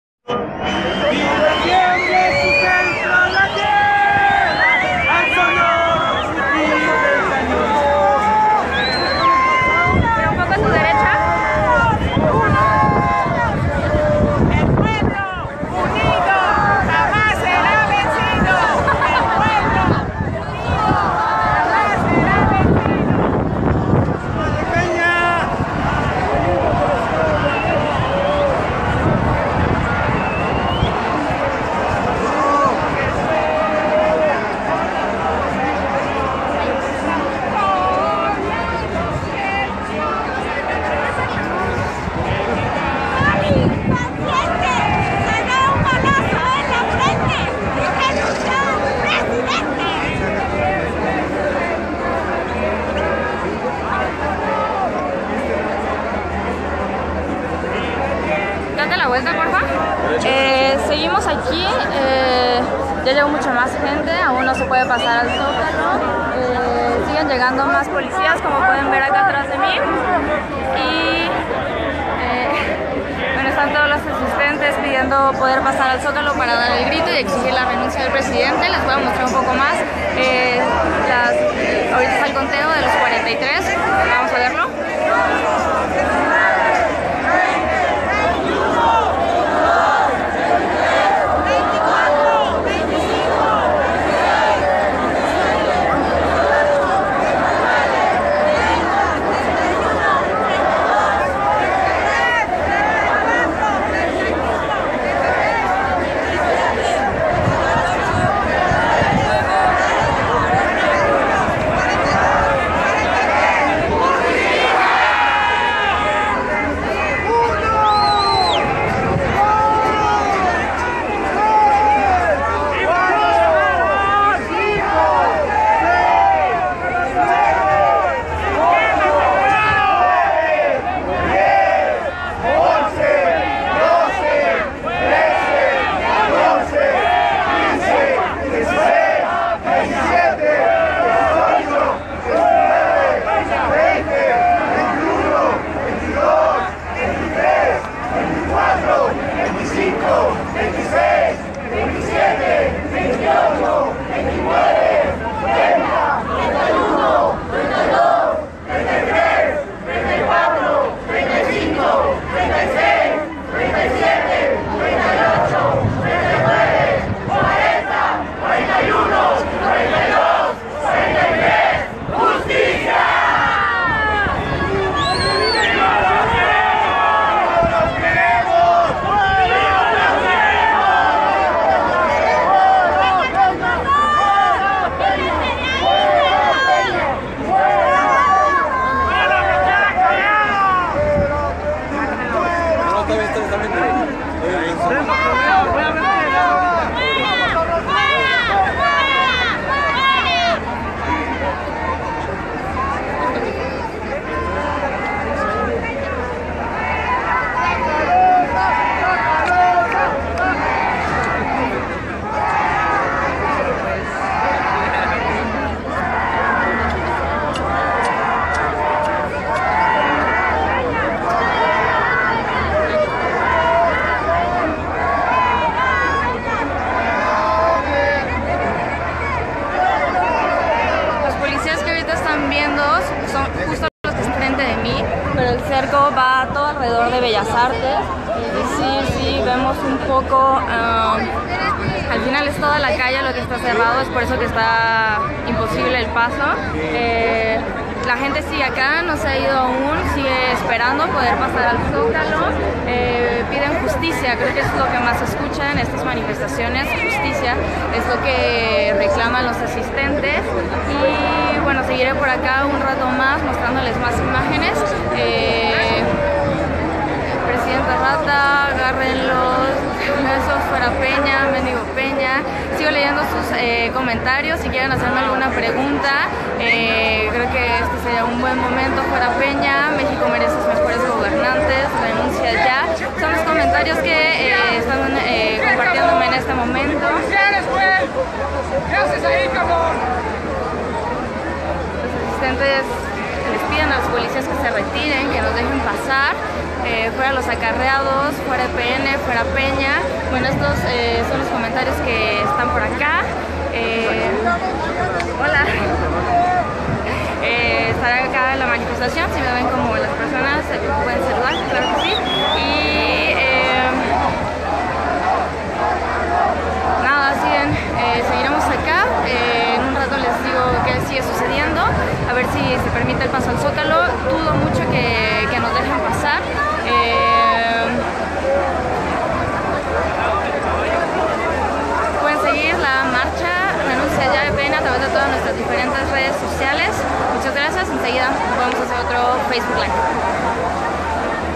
Policías detienen la marcha
Marcha convocada por la ciudadanía para manifestar su inconformidad con la administración del presidente Enrique Peña Nieto y exigir su renuncia